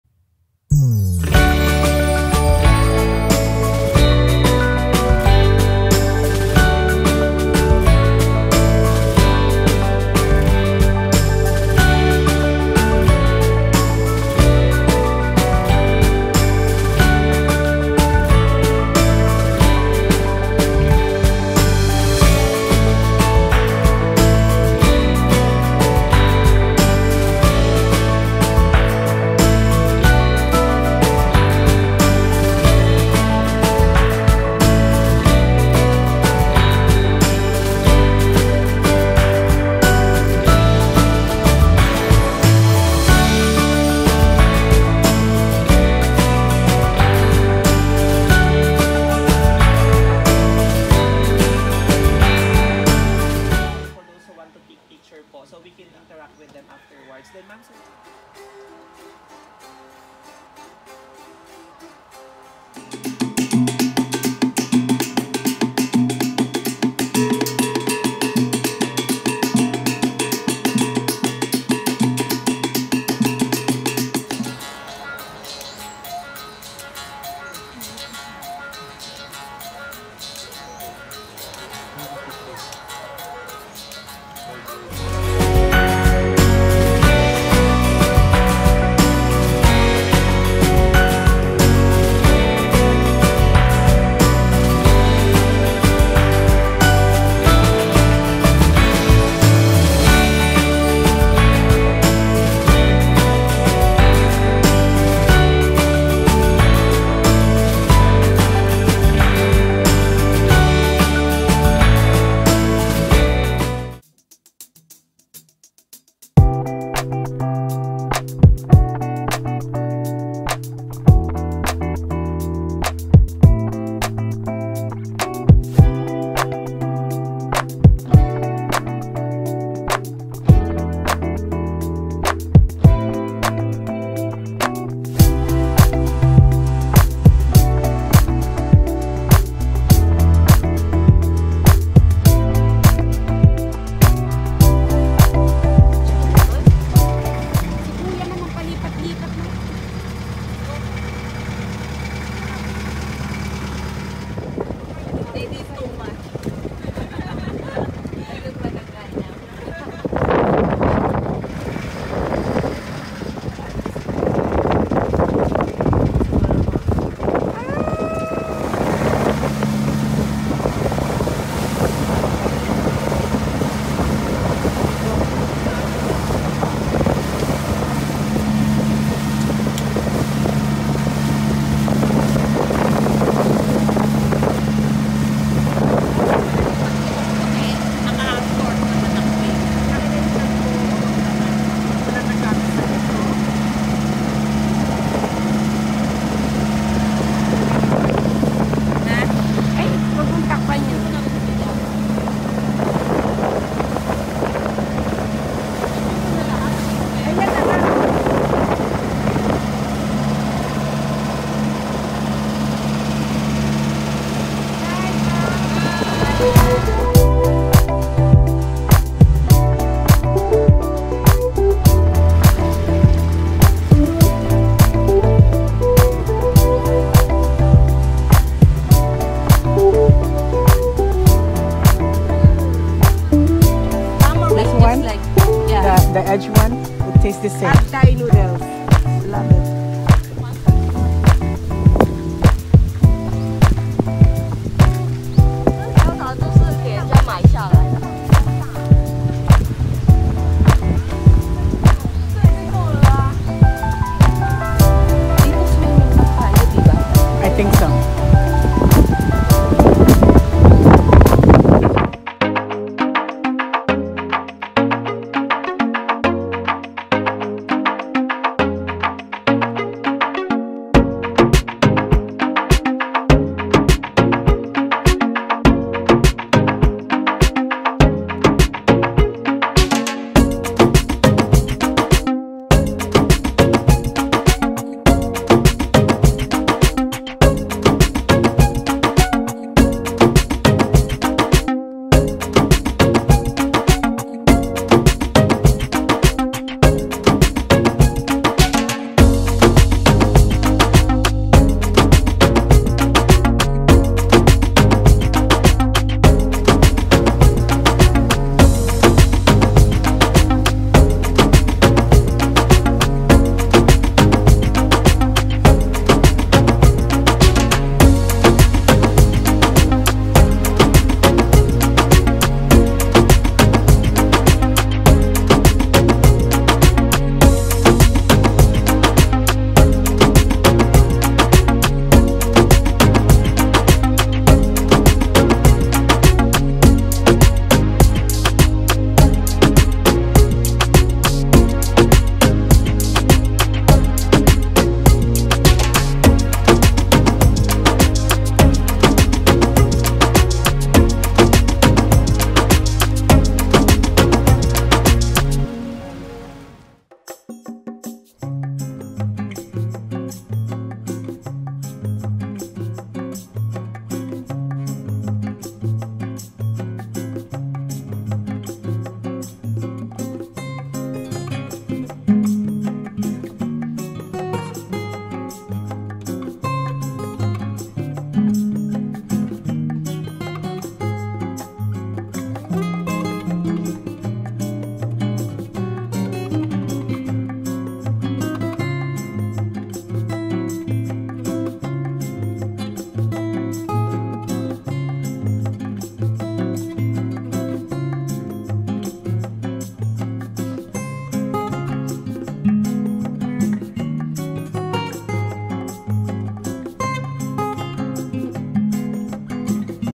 PALAWAN ESCAPADE - KEI LE
While traveling to the Philippines, I took trip to Palawan.